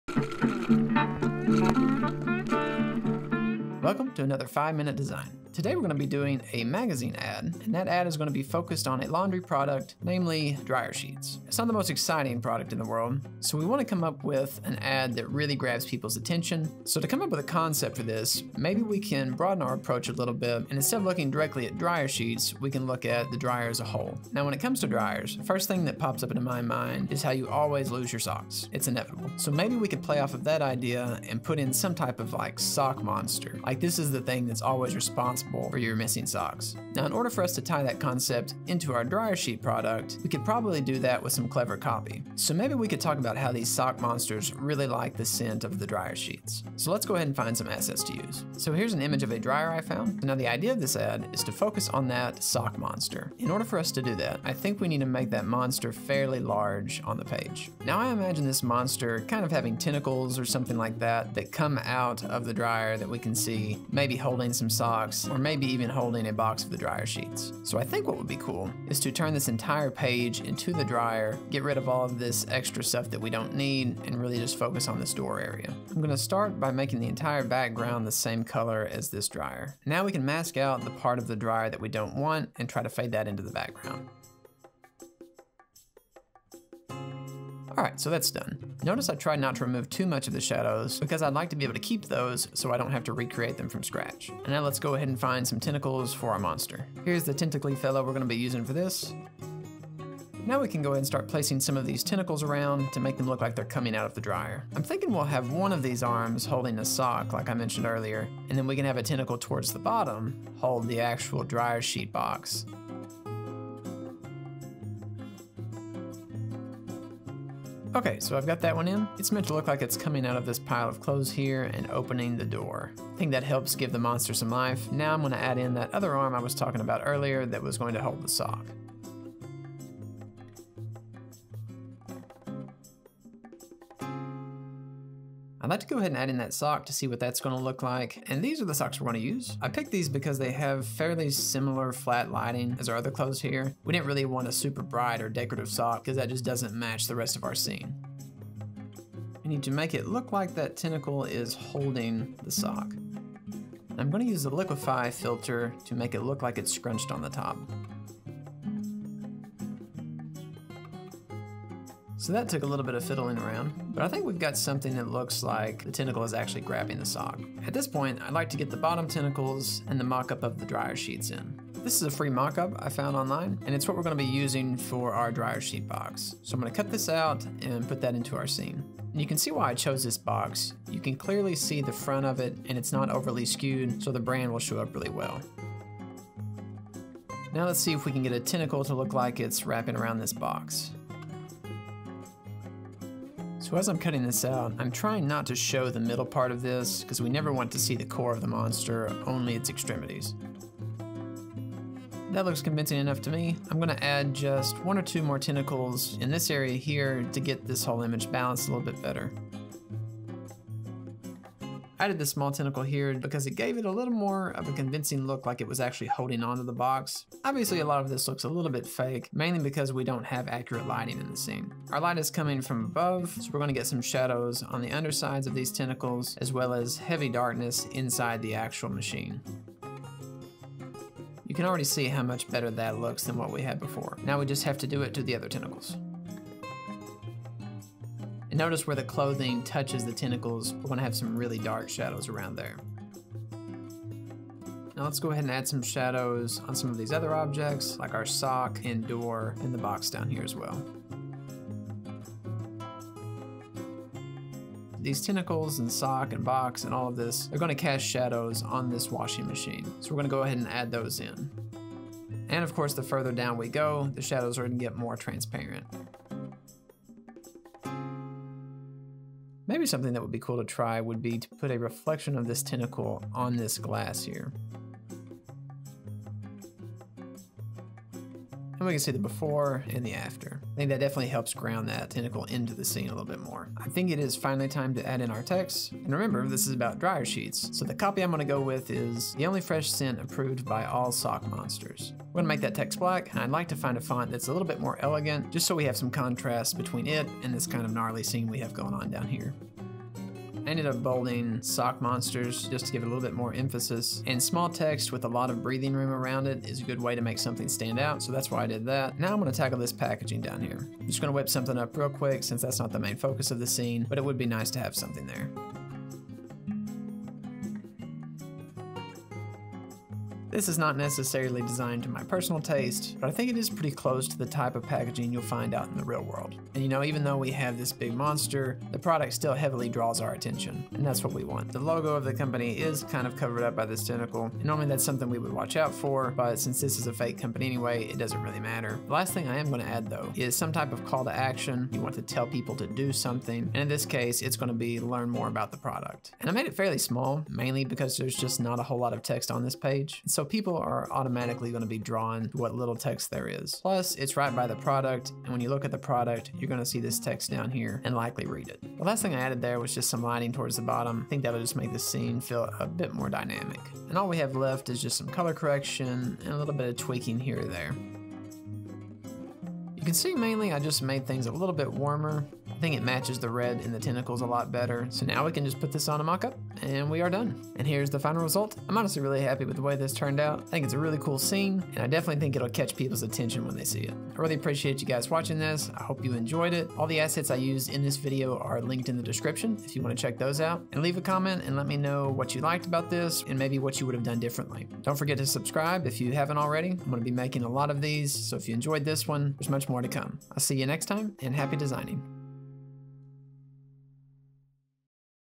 Monster Magazine Ad Design in 5 MINUTES
Follow along as I create this Monster Magazine Ad from SCRATCH in just 5 minutes (roughly)!

The Process
In 5 Minute Designs, I figure the design out as I go. I'll explain my thoughts and reasons behind the decisions I make from Concept to the Final Result. For this Monster Magazine Ad Design, I will primarily be using Adobe Photoshop CC. Let me know what you think!